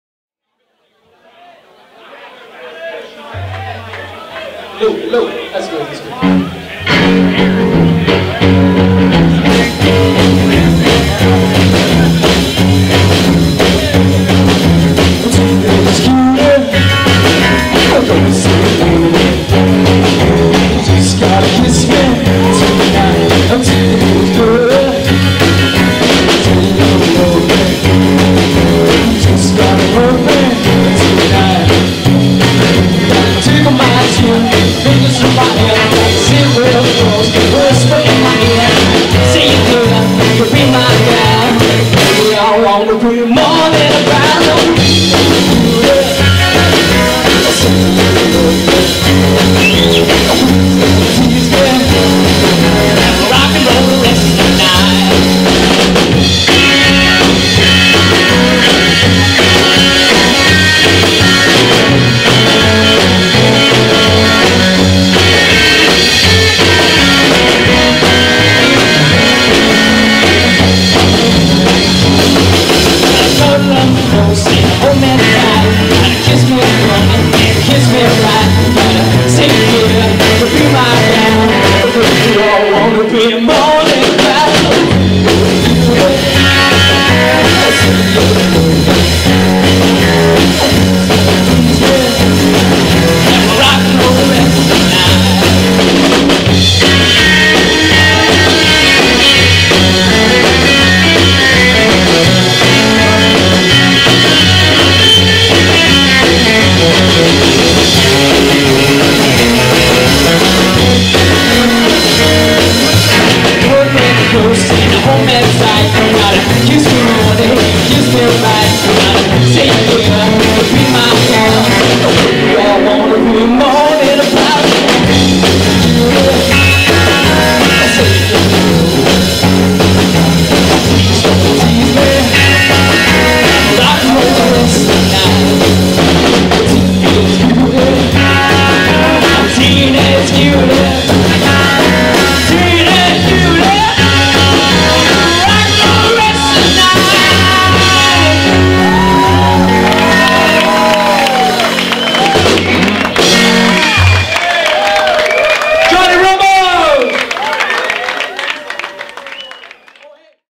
EDDIE COCHRAN HALL: JOHNNY RAMOS , Kid Ramos, Jerry Prefontaine, Buddy & Suzi
live at the EDDIE COCHRAN HALL: JOHNNY RAMOS
KID RAMOS, JERRY PREFONTAINE
BUDDY AND SUZI of HOT ROD TRIO